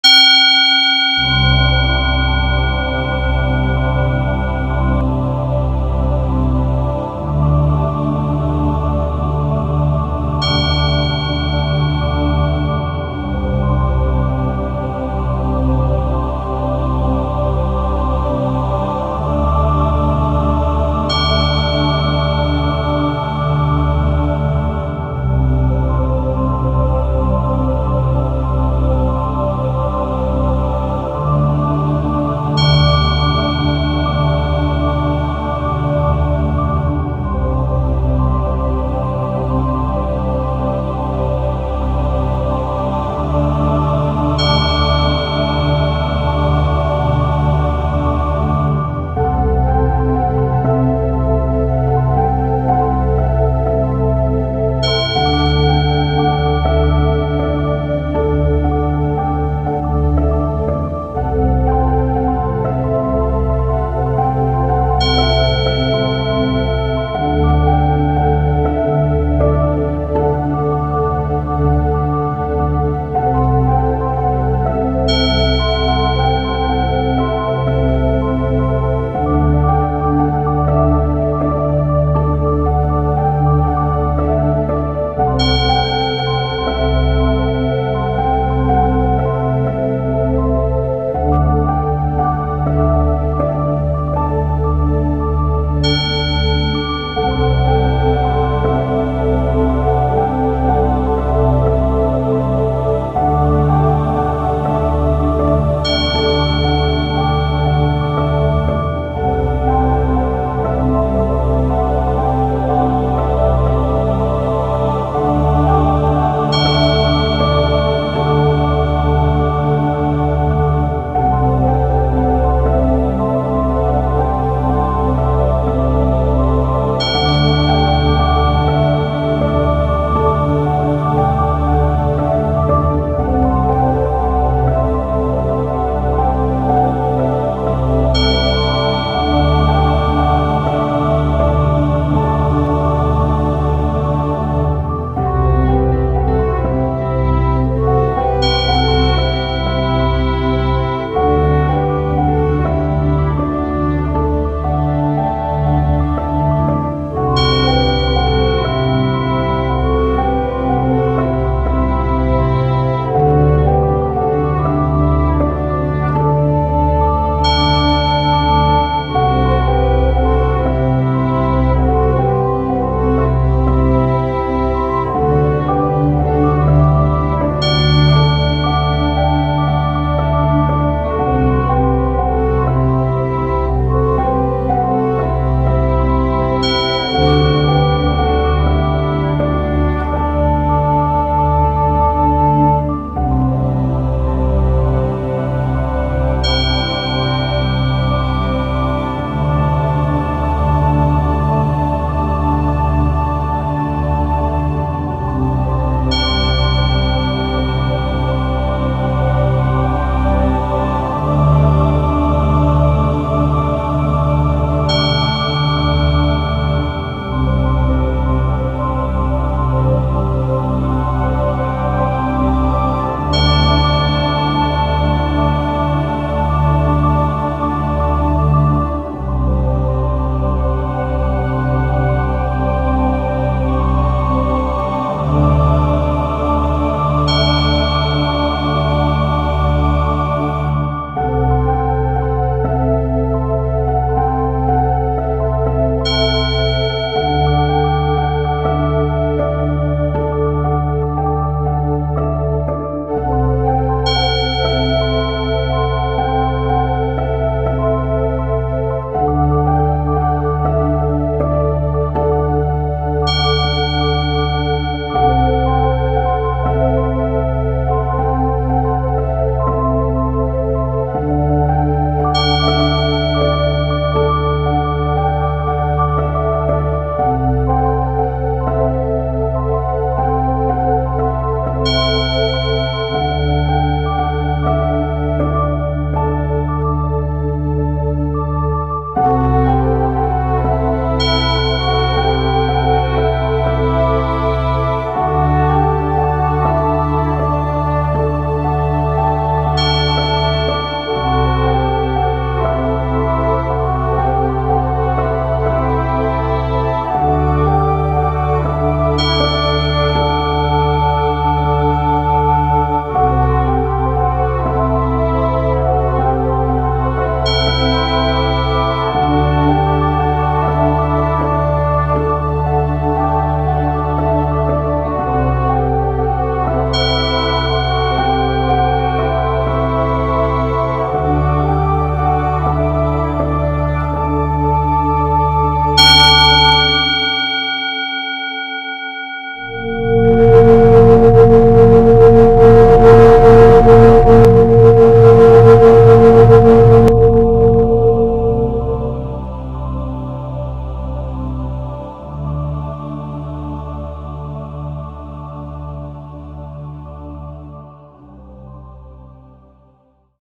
Claim It! Pakinggan araw araw upang matupad ang iyong kahilingan. Bahay at Lupa, Sasakyan at Pera.
Musikang magbibigay ng malaking pera at mga katuparan sa mga hinihiling mo. Bahay at Lupa, Sasakyan at Pera. Humiling ka ng taos sa puso at ipagkakaloob ito sa imo. Pakinggan ito araw araw ng 5minuto upang magmanifest at matupad lahat ng iyong hiling at dasal.